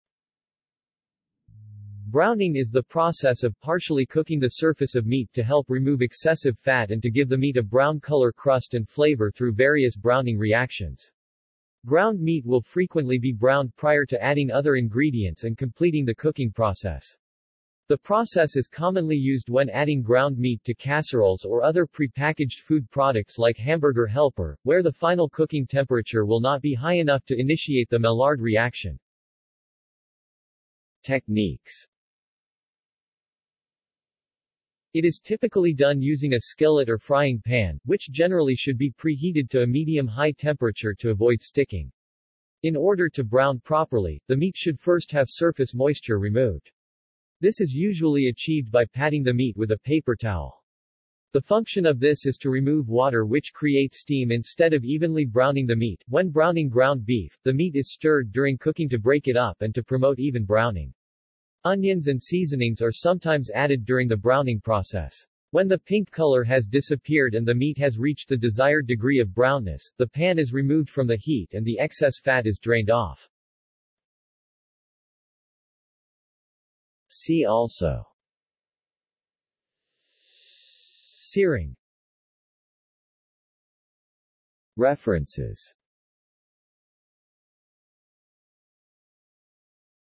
Browning (partial cooking) | Wikipedia audio article
This is an audio version of the Wikipedia Article:
Browning (partial cooking)

SUMMARY
Browning is the process of partially cooking the surface of meat to help remove excessive fat and to give the meat a brown color crust and flavor through various browning reactions. Ground meat will frequently be browned prior to adding other ingredients and completing the cooking process. The process is commonly used when adding ground meat to casseroles or other prepackaged food products like Hamburger Helper, where the final cooking temperature will not be high enough to initiate the Maillard reaction.